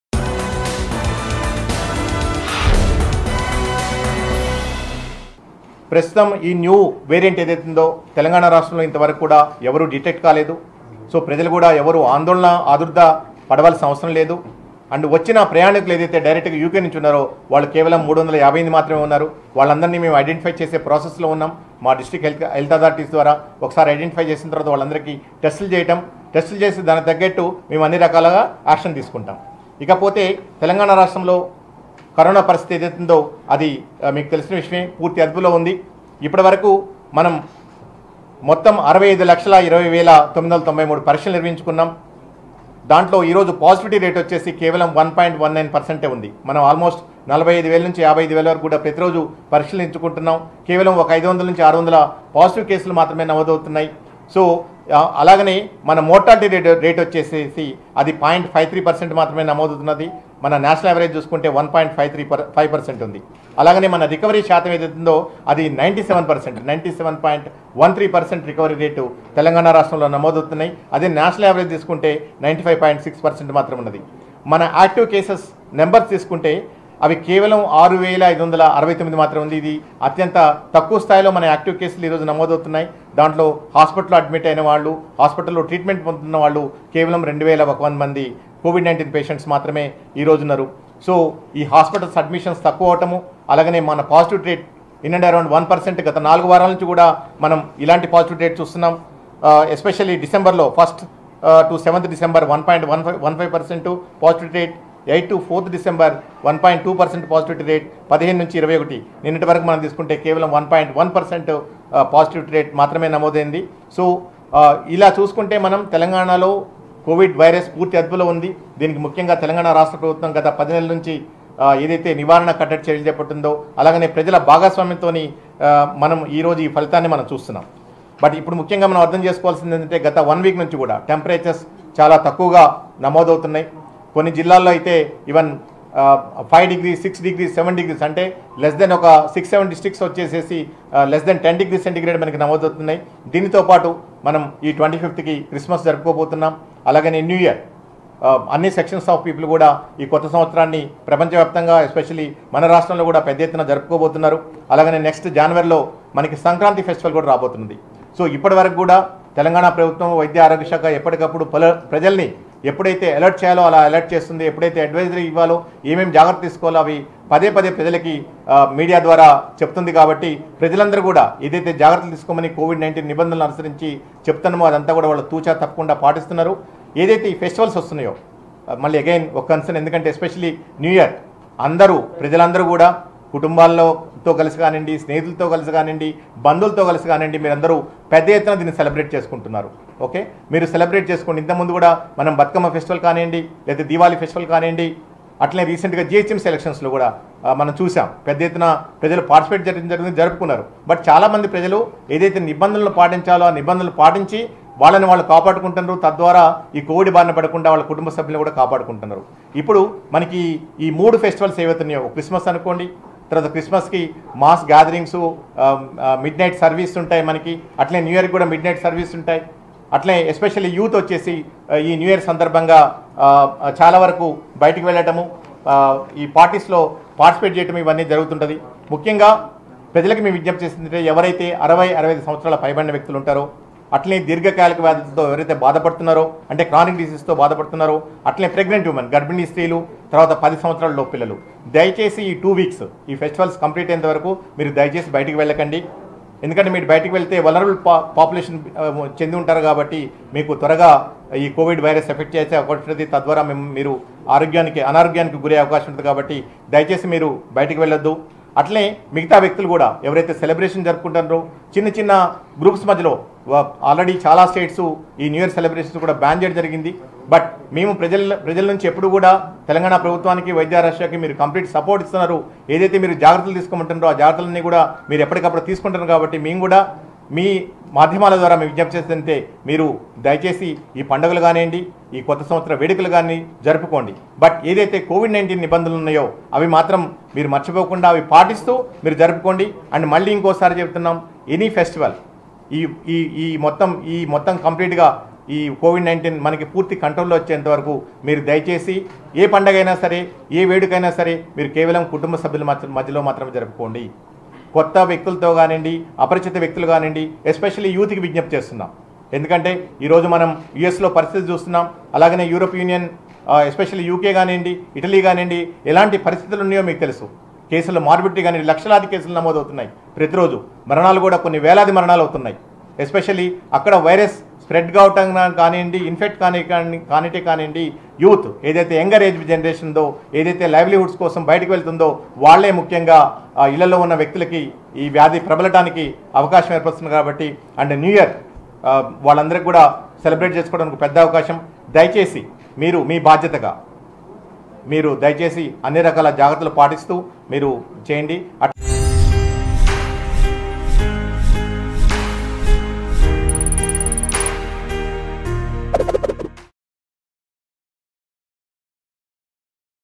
Telangana Health Director Srinivasa Rao Key Suggestions On New COVID-19 Strain | Mango News
Telangana Health Director Srinivasa Rao Key Suggestions On New COVID-19 Strain.